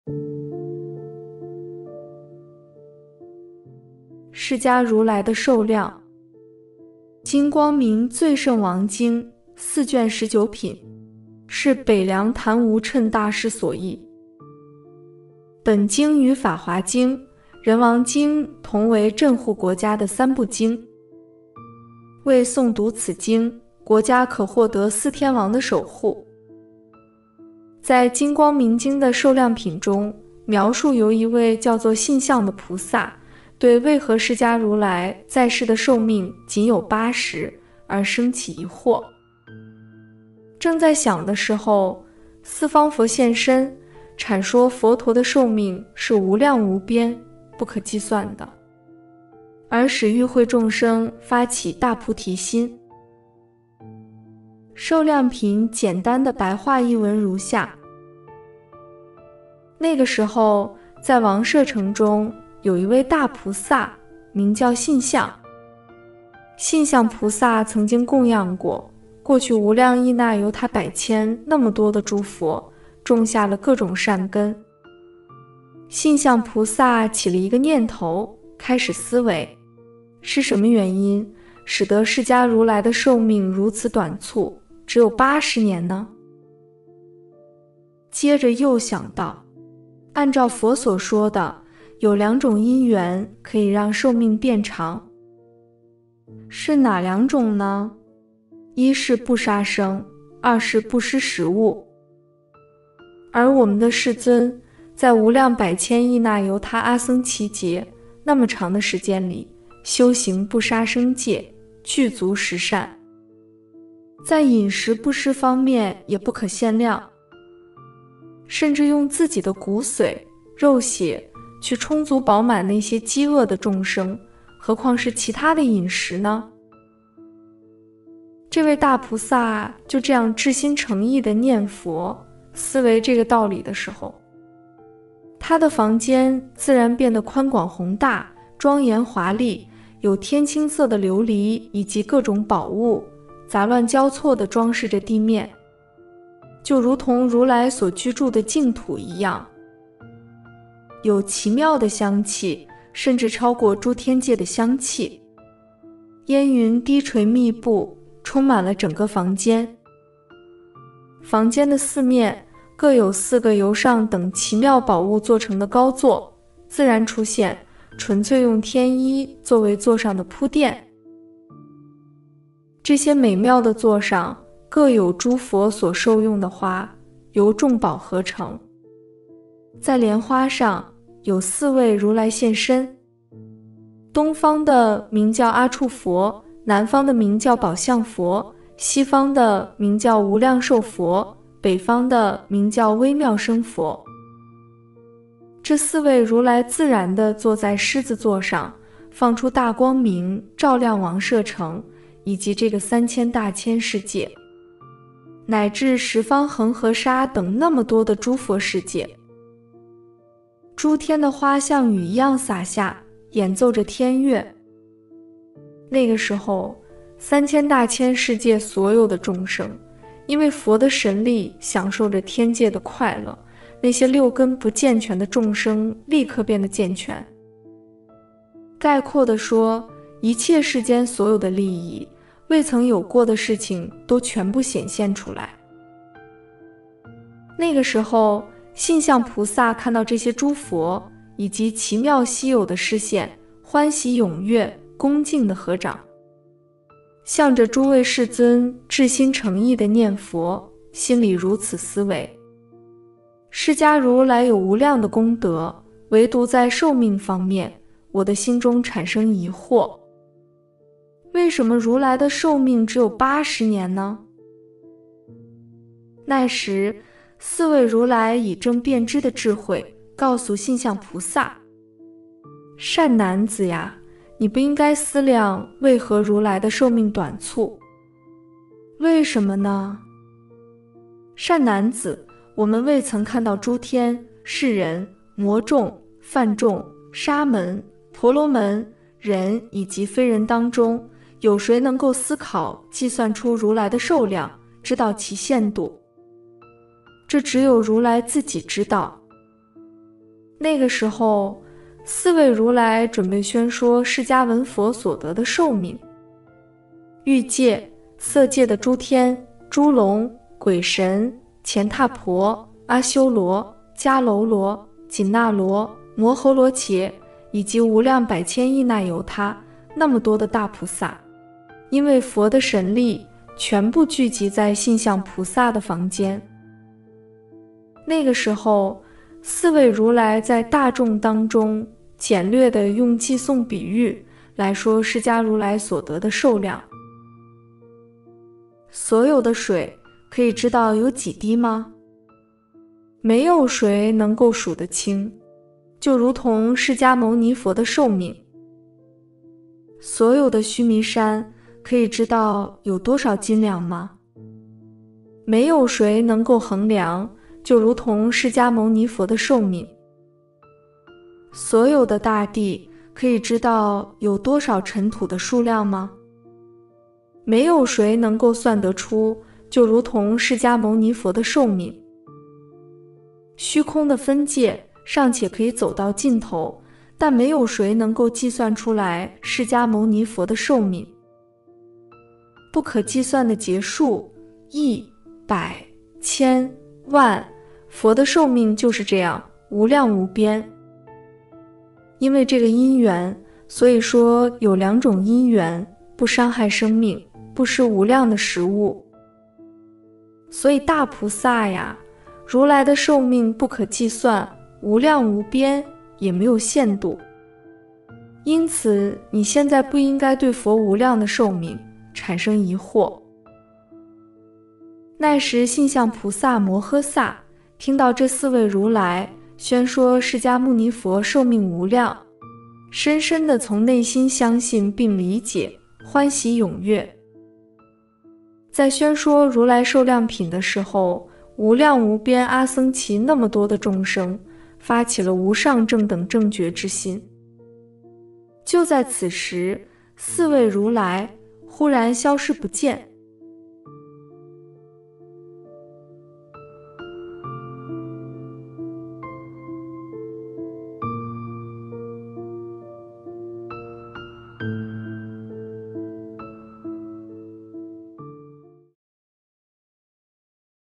【釋迦如來的壽量】
《金光明最勝王經》，四卷十九品，是北涼曇無讖大師所譯。本經與《法華經》、《仁王經》同為鎮護國家的三部經。謂誦讀此經，國家可獲得四天王的守護。在金光明經的〈壽量品〉中，描述由一位叫做「信相」的菩薩，對為何釋迦如來在世的壽命僅有八十，而生起疑惑。正在想的時候，四方佛現身，闡說佛陀的壽命是無量無邊，不可計算的，而使與會眾生發大菩提心。

〈壽量品〉簡單的白話譯文如下 ：

 那個時候在王舍城中，有一位大菩薩名叫「信相」。信相菩薩曾經供養過過去無量億那由他百千那麽多的諸佛，種下了各種善根。信相菩薩起了一個念頭，開始思惟：是什麽原因，使得釋迦如來的壽命如此短促，只有八十年呢？

 接著又想到：按照佛所說的，有兩種因緣可以讓壽命變長。是哪兩種呢？一是不殺生，二是布施食物。而我們的世尊，在無量百千億那由他阿僧祇劫，那麽長的時間裏，修行不殺生戒，具足十善，在飲食布施方面也不可限量。甚至用自己的骨髓、肉血，去充足飽滿那些饑餓的衆生，何況是其他的飲食呢。

 這位大菩薩就這樣至心誠意地念佛，思惟這個道理的時候，他的房間自然變得寬廣宏大、莊嚴華麗，有天青色的琉璃以及各種寶物，雜亂交錯地裝飾著地面，就如同如來所居住的淨土一樣。有奇妙的香氣，甚至超過諸天界的香氣，煙雲低垂密布，充滿了整個房間。房間的四面，各有四個由上等奇妙寶物做成的高座自然出現，純粹用天衣作為座上的鋪墊。

 這些美妙的座上各有諸佛所受用的花，由衆寶合成。在蓮花上有四位如來現身。東方的名叫阿閦佛，南方的名叫寶相佛，西方的名叫無量壽佛，北方的名叫微妙聲佛。這四位如來自然地坐在獅子座上，放出大光明照亮王舍城，以及這個三千大千世界，乃至十方恒河沙等那麽多的諸佛世界，諸天的花像雨一樣灑下，演奏著天樂。那個時候，三千大千世界所有的衆生，因為佛的神力享受著天界的快樂，那些六根不健全的衆生立刻變得健全。概括地說，一切世間所有的利益、未曾有過的事情都全部顯現出來。

 那個時候，信相菩薩看到這些諸佛以及奇妙稀有的示現，歡喜踴躍，恭敬地合掌，向著諸位世尊至心誠意地念佛，心中如此思惟：釋迦如來有無量的功德，唯獨在壽命方面，我的心中產生疑惑。為什麽如來的壽命只有八十年呢？

 那個時候，四位如來以正遍知的智慧告訴信相菩薩：「善男子呀，你不應該思量為何如來的壽命短促。為什麽呢？善男子，我們未曾看到諸天、世人、魔衆、梵衆、沙門、婆羅門、人以及非人當中有誰能夠思考計算出如來的壽量，知道其限度。這只有如來自己知道。」
   那時，四位如來準備宣說釋迦文佛所得的壽命，欲界、色界的諸天、諸龍、鬼神、乾闥婆、阿修羅、迦樓羅、緊那羅、摩睺羅伽，以及無量百千億那由他那麽多的大菩薩，因為佛的神力，全部聚集在信相菩薩的房間。那個時候，四位如來在大衆當中，簡略地用偈頌比喻，來說釋迦如來所得的壽量。

 所有的水，可以知道有幾滴嗎？沒有誰能夠數得清，就如同釋迦牟尼佛的壽命。所有的須彌山，可以知道有多少斤兩嗎？沒有誰能夠衡量，就如同釋迦牟尼佛的壽命。所有的大地，可以知道有多少塵土的數量嗎？沒有誰能夠算得出，就如同釋迦牟尼佛的壽命。虛空的分界，尚且可以走到盡頭，但沒有誰能夠計算釋迦牟尼佛的壽命。不可計算的劫數，億、百、千、萬，佛的壽命就是這樣，無量無邊。因為這個因緣，所以說有兩種因緣，不傷害生命、布施無量的食物。所以大菩薩呀，如來的壽命不可計算，無量無邊，也沒有限度。因此你現在不應該對佛無量的壽命產生疑惑。

 那時信相菩薩摩訶薩，聽到這四位如來，宣說釋迦牟尼佛壽命無量，深深地從內心相信並理解，歡喜踴躍。在宣說如來壽量品的時候，無量無邊阿僧祇那麽多的衆生，發起了無上正等正覺之心。就在此時，四位如來忽然消失不見。